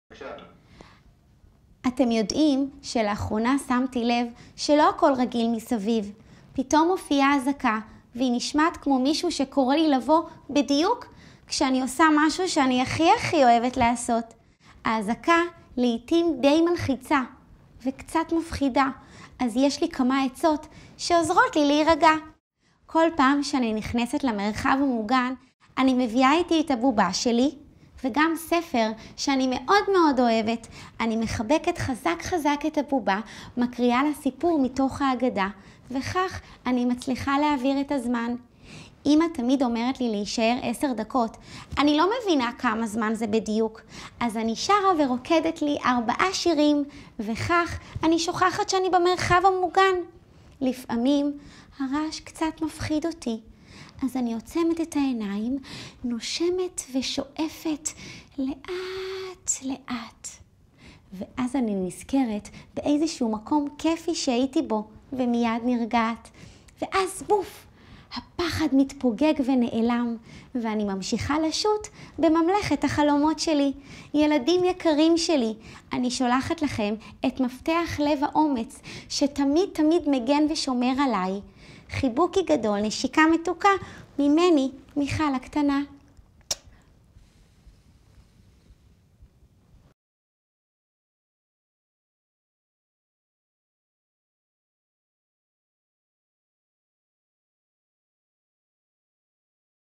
מיכל מרגיעה את כל הילדים המודאגים
מיכל נרתמת לפיקוד העורף.